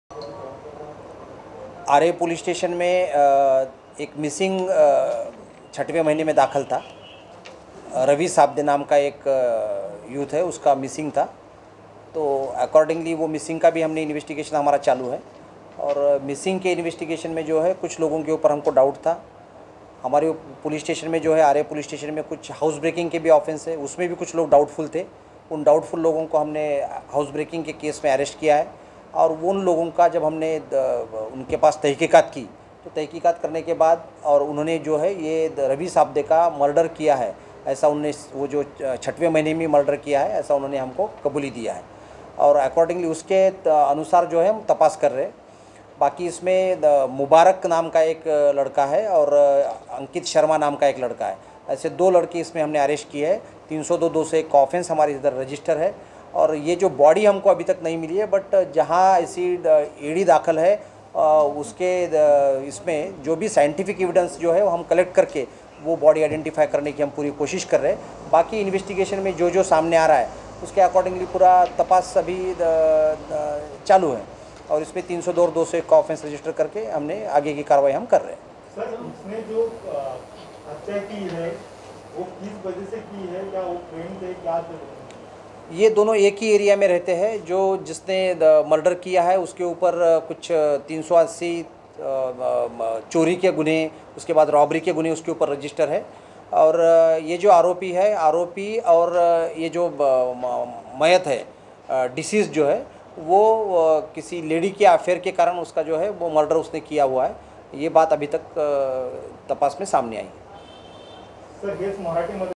Mumbai Aarrey Police_चोर निकला हत्यारा
प्यार में दरार पड़ने के बाद महिला के दूसरे प्रेमी ने की पहले प्रेमी की हत्या